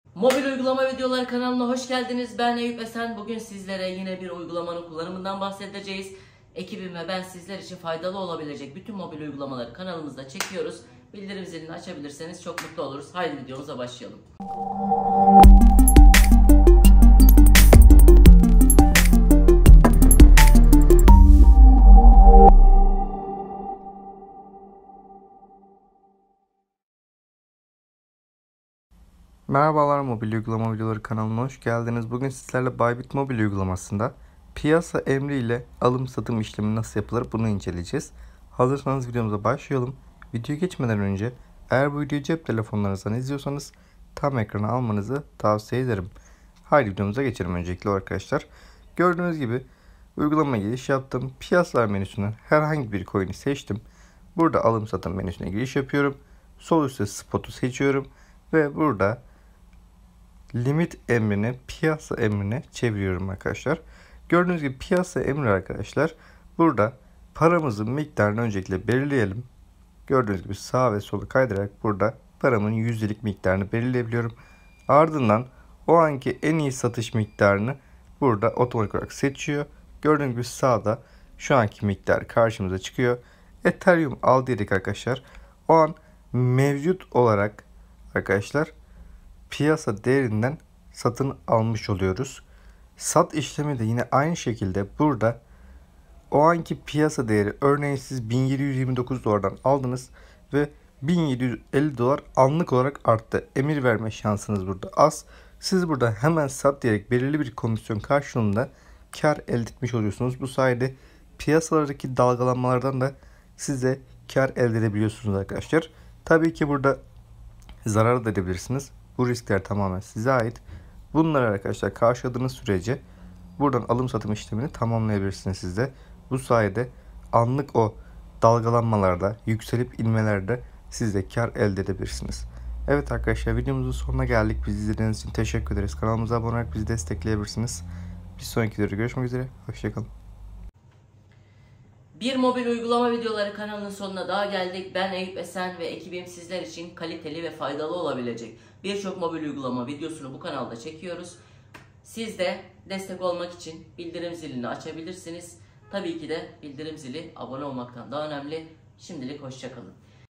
Bybit Mobil Uygulama Piyasa Alım-Satım
Gate StartUp Kazanmak İçin Kayıt Linki (Detaylı sermayesiz AirDrop video linki yanda) Bybit, kripto para borsalarından biridir ve kullanıcılarına güvenli ve hızlı işlem yapma imkanı sunan bir platformdur. Bybit mobil uygulaması ise kullanıcıların her yerden kolayca işlem yapabilmesini sağlayan bir araçtır. Bybit, kripto para türevleri üzerinde işlem yapmak isteyen kullanıcılar için geliştirilmiş bir platformdur. Kullanıcılar, Bitcoin, Ethereum, Ripple gibi popüler kripto para birimleri üzerinde vadeli işlem yapabilirler. Bybit, güvenliği, likiditesi ve hızlı işlem imkanıyla tanınan bir borsadır.

Piyasa emri, kullanıcıların anlık olarak kripto para birimleri alım veya satım işlemi yapmasını sağlayan bir emir türüdür. Piyasa emri verildiğinde, işlem anında gerçekleştirilir ve mevcut piyasa fiyatından alım veya satım yapılır. Piyasa emri, hızlı ve pratik bir şekilde işlem yapmak isteyen kullanıcılar için ideal bir seçenektir. Bybit mobil uygulamasında Piyasa Emri Menüsü, kullanıcıların anlık olarak piyasa fiyatından işlem yapabileceği bir bölümdür. Kullanıcılar, Bybit mobil uygulamasındaki Piyasa Emri Menüsü üzerinden istedikleri kripto para birimini anlık olarak alabilir veya satabilirler. Piyasa emri verildiğinde, işlem hemen gerçekleşir ve kullanıcının portföyü güncellenir. Piyasa emri verirken, kullanıcılar istedikleri işlem boyutunu belirleyebilirler. Bu, kullanıcılara esneklik sağlar ve kendi risk yönetimi stratejilerine göre işlem yapma imkanı sunar. Bybit mobil uygulamasındaki Piyasa Emri Menüsü, kullanıcılara güncel piyasa fiyatlarını sağlar. Kullanıcılar, anlık fiyat bilgilerini gözlemleyerek daha bilinçli işlem kararları alabilirler.

Sonuç olarak, Bybit mobil uygulamasının Piyasa Emri Menüsü, kullanıcıların anlık olarak kripto para birimleri alım veya satım işlemi yapmalarını sağlayan bir bölümdür. Kullanıcılar, hızlı, güvenli ve kullanıcı dostu bir arayüz üzerinden piyasa emri vererek kripto para ticaretini gerçekleştirebilirler. Bybit'in Piyasa Emri Menüsü, yatırımcıların daha hızlı ve pratik işlem yapmalarını destekler ve kullanıcı deneyimini artırır.
🤖 Android Mobil Uygulama Linki:
🍏 IOS Mobil Uygulama Linki: